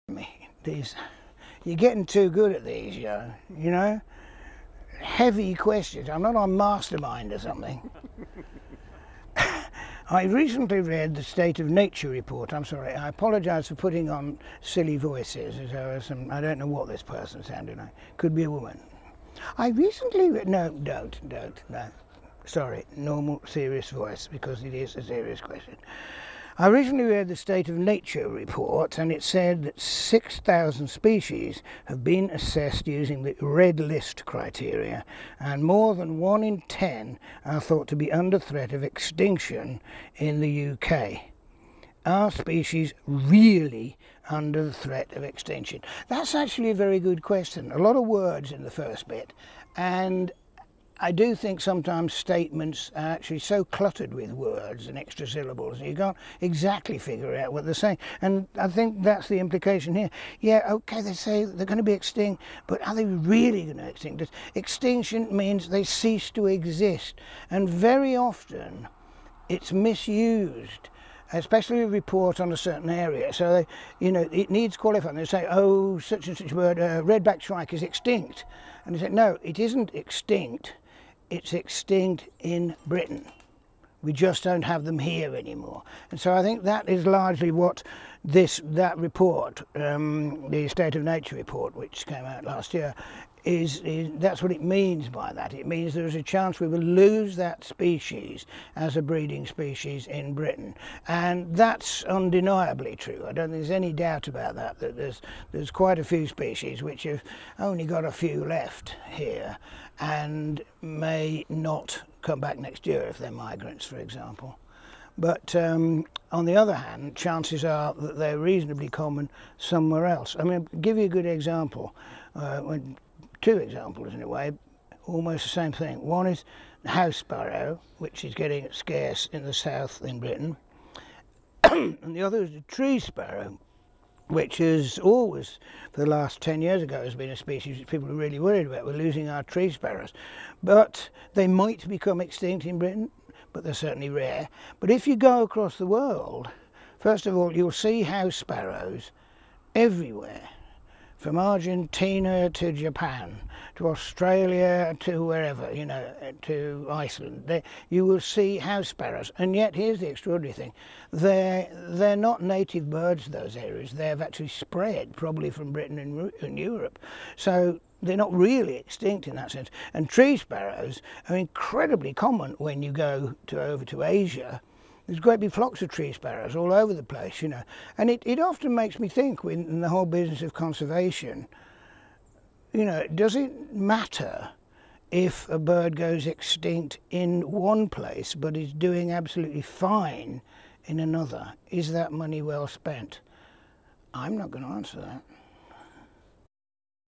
Are species REALLY under threat of extinction?
The AskBillOddie questions didn't get any easier for Bill this month: "I recently read The State of Nature report and it said that 6,000 species have been assessed using the "Red List" criteria, and more than one in 10 are thought to be under threat of extinction in the UK. Are species REALLY under threat of extinction?"